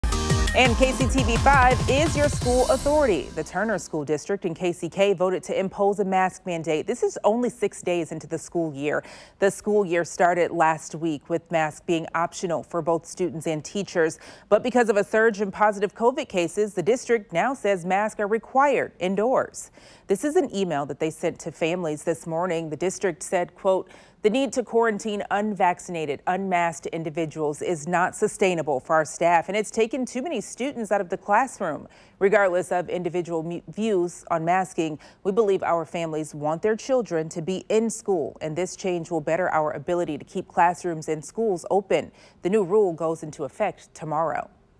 Turner School District votes to impose mask mandate 6 days into new school year
The Turner School District has voted to impose a mask mandate just six days into the school year.  The school year started last week with masks optional for students and teachers. But due to a surge in positive COVID cases in the district, masks will now be required indoors.